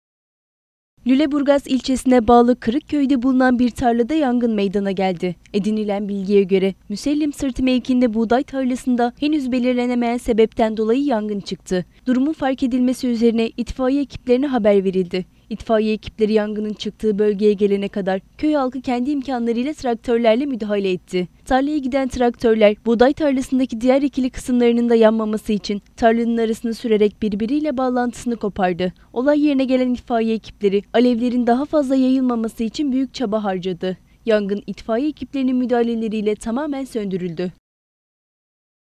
LÜLEBURGAZ’DA TARLA YANGINI KORKUTTU
Kırklareli'nin Lüleburgaz ilçesine bağlı Kırıkköy'de buğday tarlasında çıkan yangın, vatandaşların ve itfaiyenin erken müdahalesiyle söndürüldü.